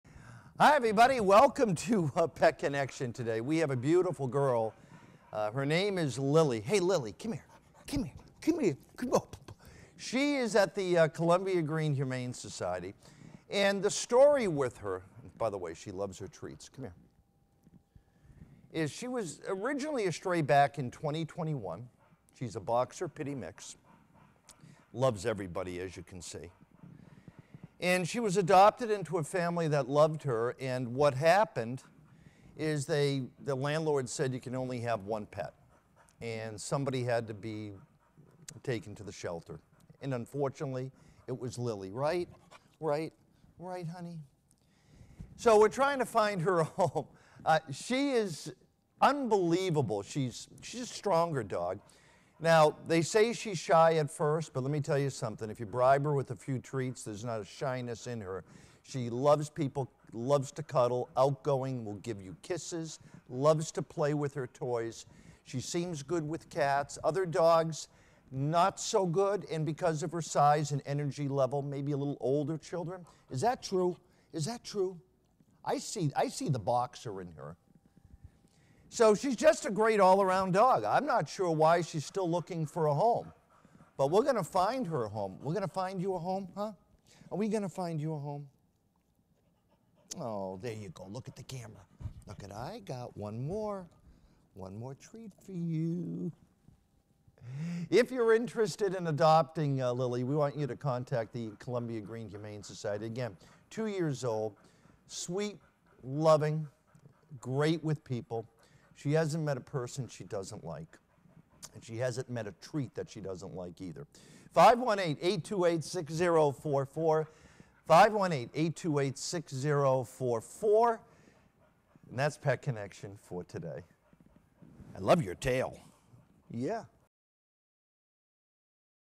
Pet Connection Lily 7-19-23
I needed this-A LOVE Bug to the Max !
"Lily" is so sweet...gives you butterfly kisses !
She had a wonderful home but unfortunately she lost it. not her fault at all. Lets get her a home where she can LOVE You !

Pet Connection 7-19-23

Lily is a 4 and a half year old Boxer/Pitbull mix. She was initially found as a stray in 2021; she was actually on the run for about 2 weeks before she was caught.

Lily was adopted from us shortly after arriving in 2021. After almost 2 years, she was returned because of landlord issues; they only allowed one pet and they unfortunately did not choose Lily.

She's a complete sweetheart and loves everyone she meets. She can be a little shy when she meets someone, but after a few minutes she really opens up and you can see just how sweet she is.

Lily does like to play, but she really prefers human contact over toys. She's good with cats but unfortunately cannot live with other dogs. Because of her slight shyness, we believe older kids are best for her.

Columbia Greene Humane Society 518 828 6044